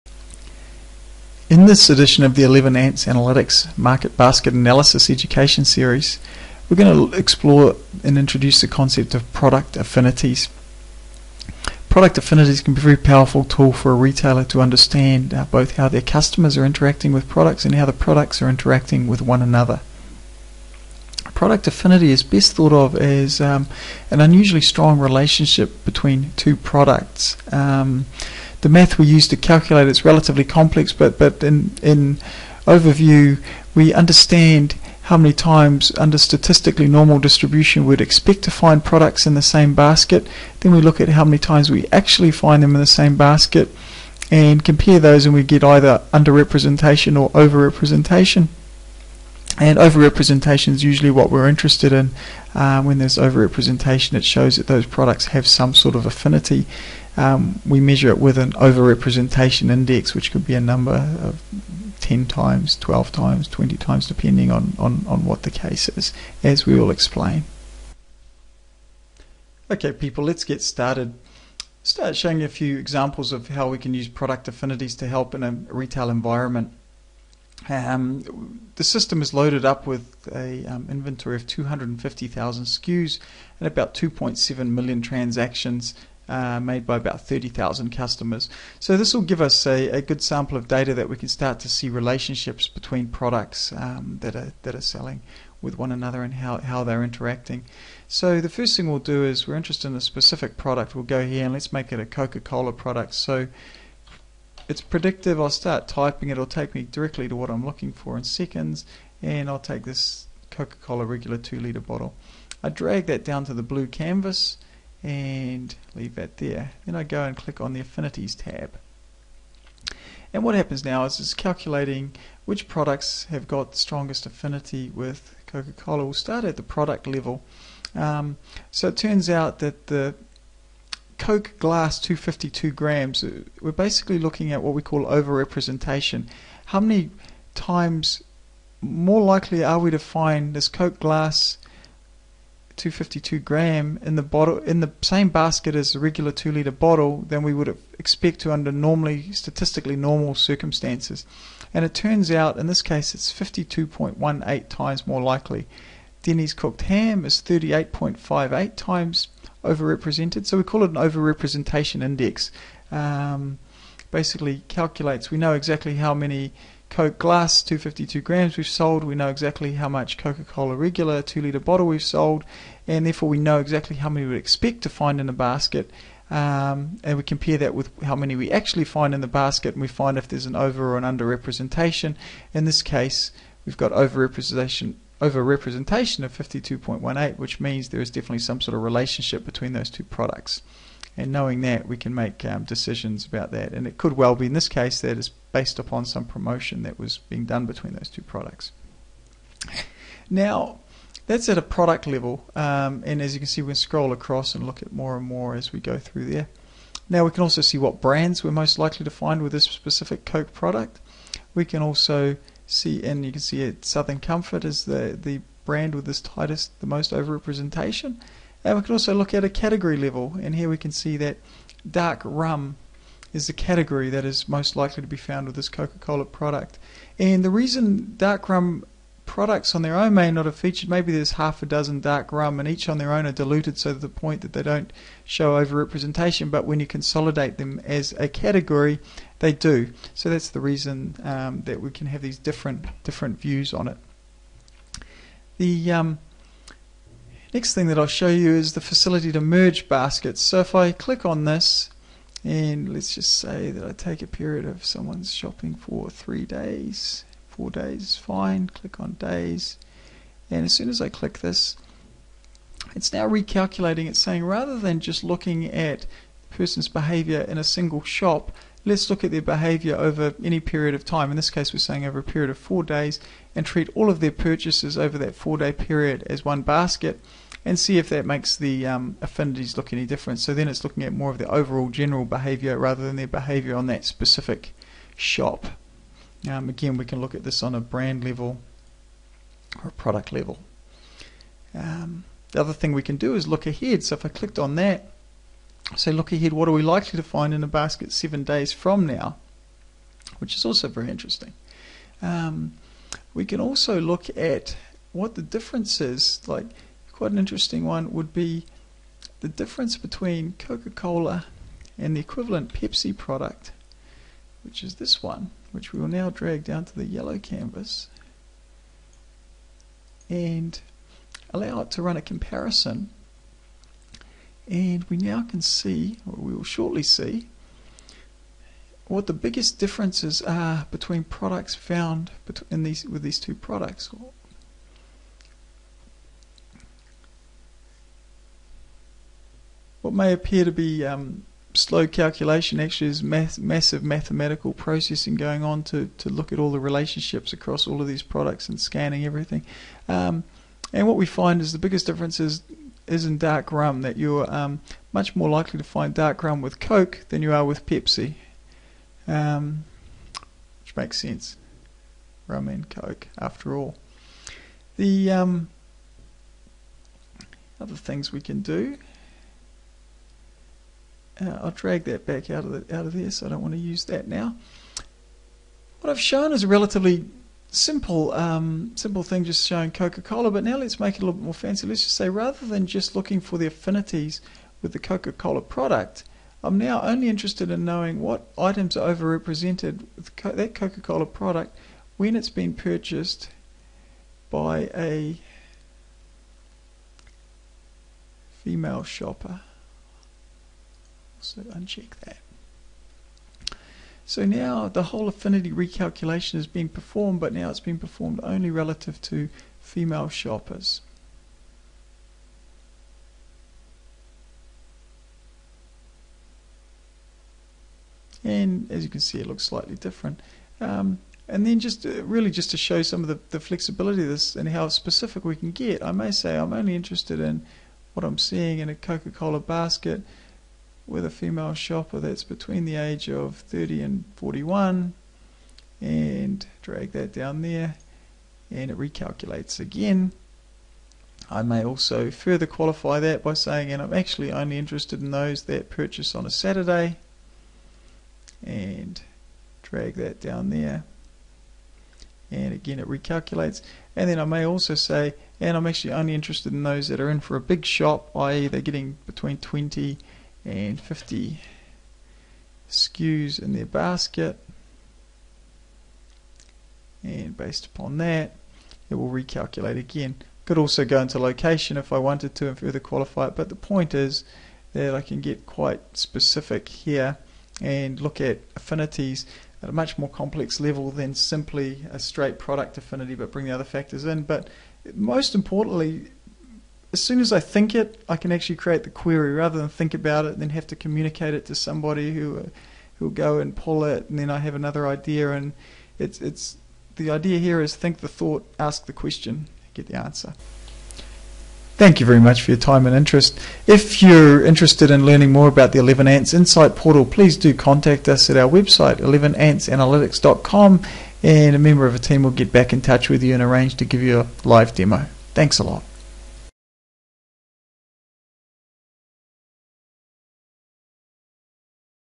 Product Affinities - Market Basket Analysis Education Series by 11Ants Analytics
This edition of the 11Ants Analytics Market Basket Analysis Education Series introduces the concept of PRODUCT AFFINITIES. Knowledge of product affinities can be a very powerful tool for retailers, in helping them understand both how their customers are buying products and how their products are interacting together. The video deals with the question 'what is a product affinity?'. It then demos various ways affinities can be queried using several FMCG examples. The solution used to demonstrate the concepts is the 11Ants Insight Portal - an advanced market basket analysis solution.